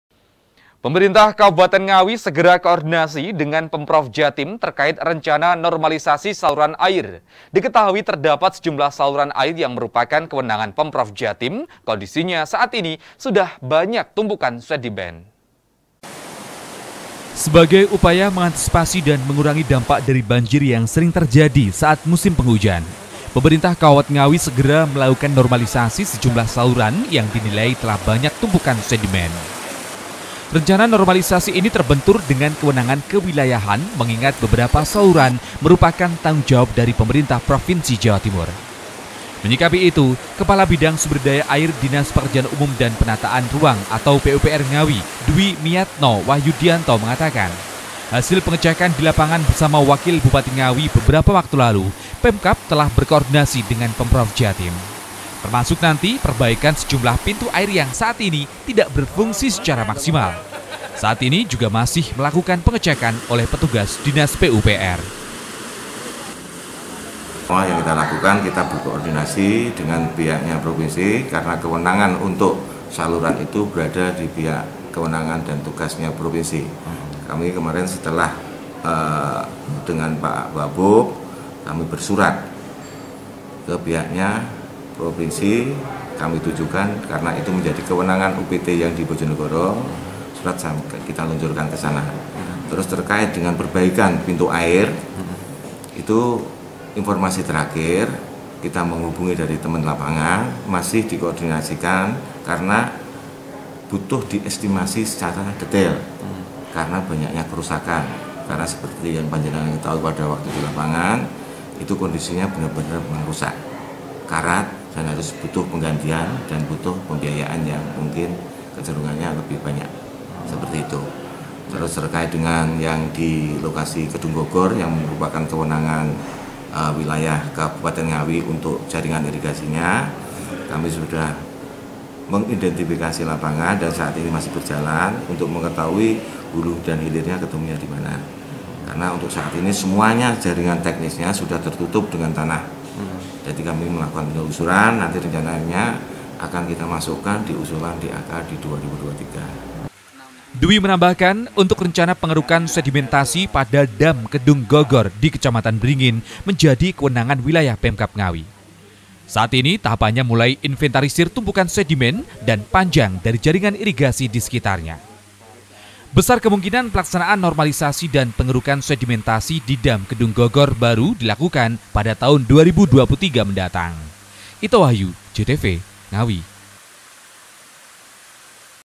NGAWI - PEMKAB Koordinasi Dengan Pemprov Jatim Untuk Normalisasi Saluran Air
Pemerintah kabupaten ngawi segera koordinasi dengan pemprov jatim terkait rencana normalisasi saluran air. Diketahui terdapat sejumlah saluran air yang merupakan kewenangan pemprov jatim kondisinya saat ini sudah banyak tumpukan sedimen. Hal itu kerap mengakibatkan air hujan menggenangi pemukiman dan jalan utama ngawi-caruban.